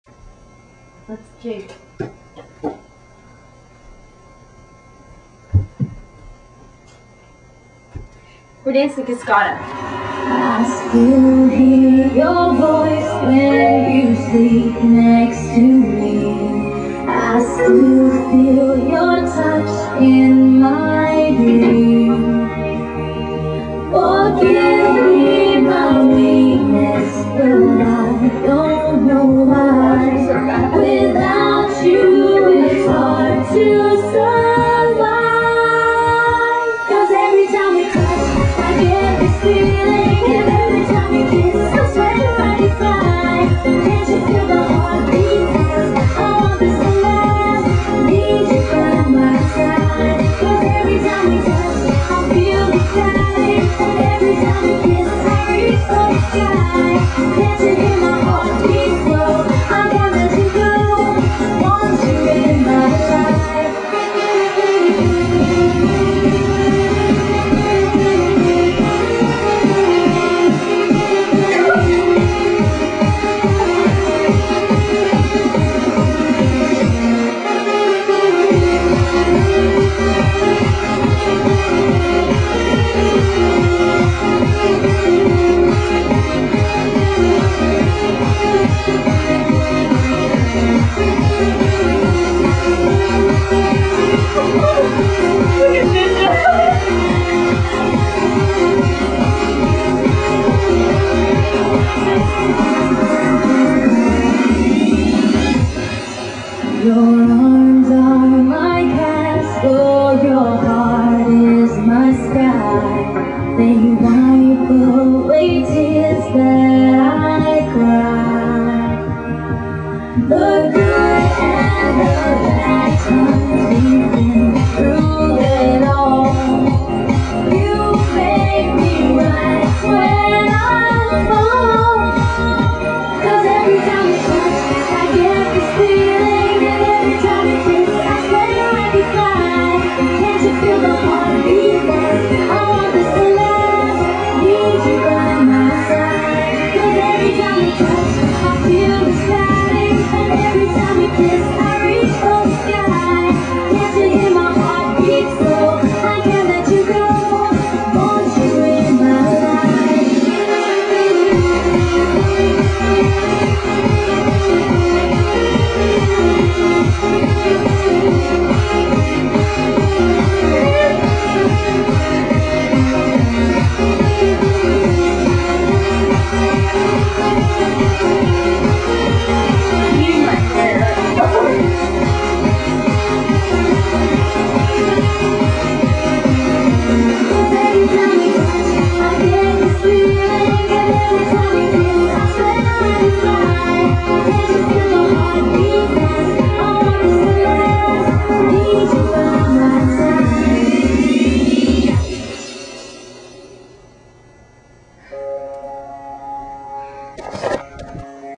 Cascada
us dancing to the infamous cascada